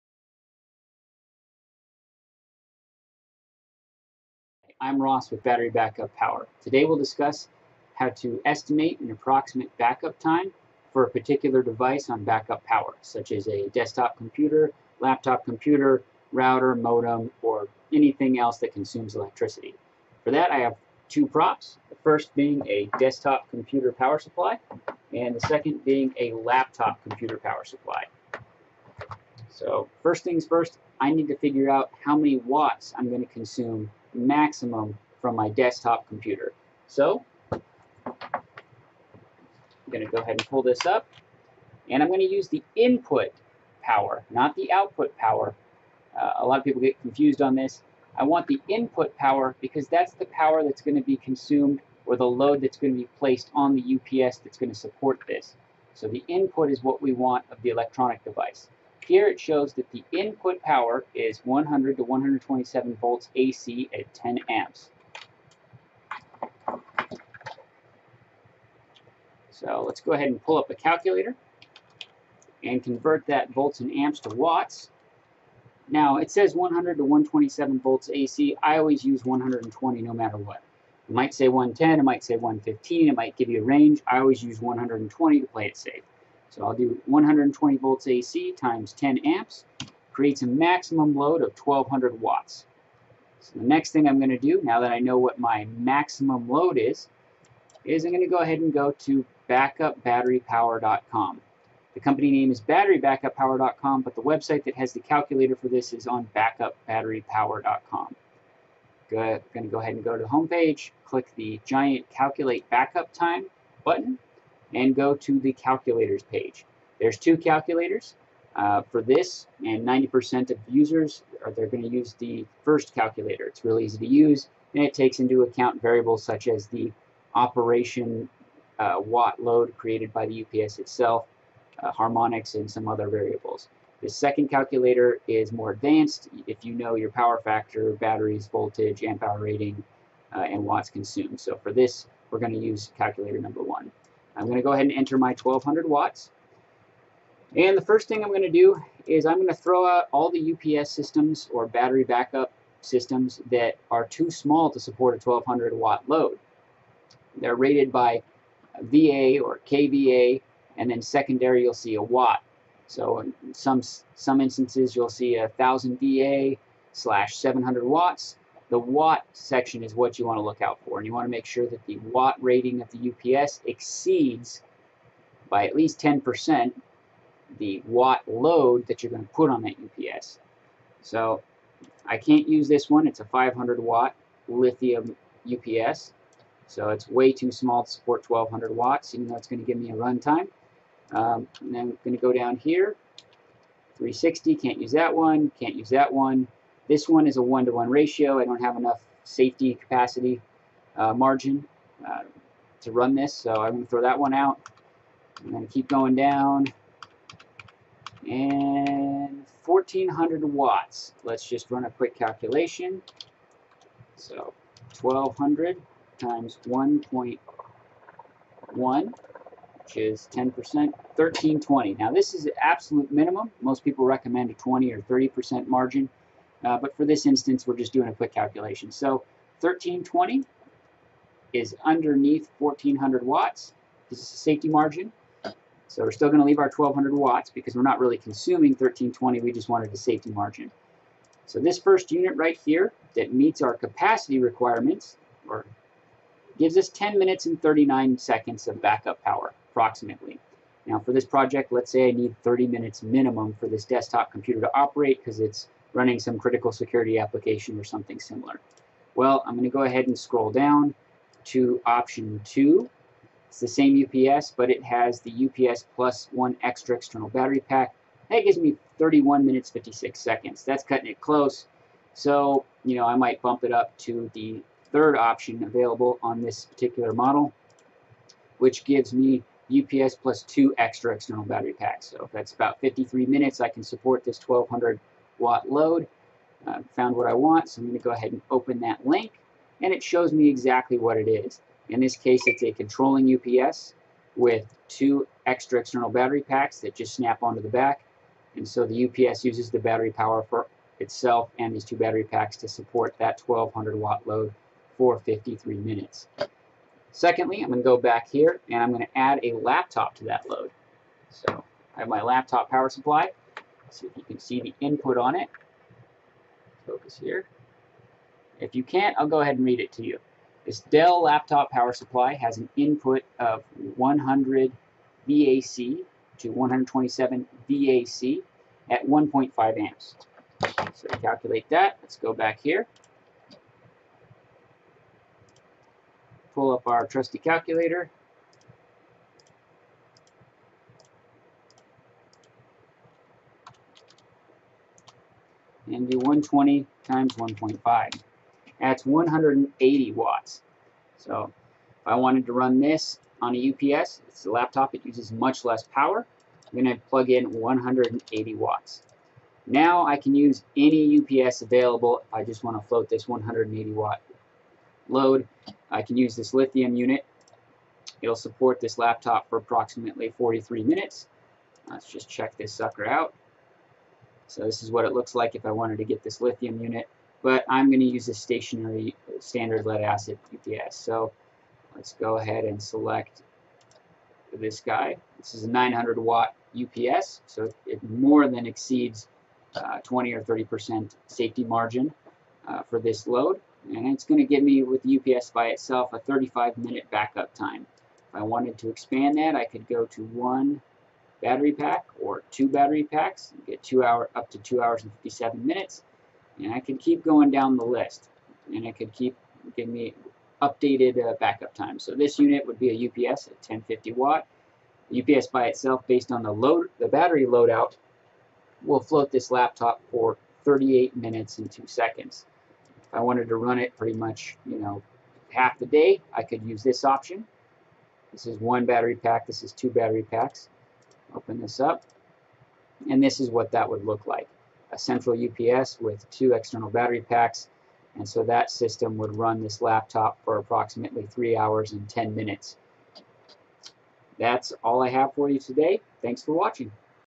How To Calculate Battery Backup Time In 2 Steps
This video explains how to calculate your approximate battery backup time for certain Battery Backup Power, Inc. UPS systems in 2 easy steps located

Step 1: Enter your electrical load in watts.

Step 2: Use the drop down menu to browse the estimated backup time for the watt load you entered on Step 1 on different UPS models.

The calculator used in the video takes into account factors such as battery voltage, amp hour rating of batteries, DC BUS voltage, inverter efficiency,  power factor, number of batteries, power consumed by the Battery Backup UPS while operating, and many other variables so you don't have to.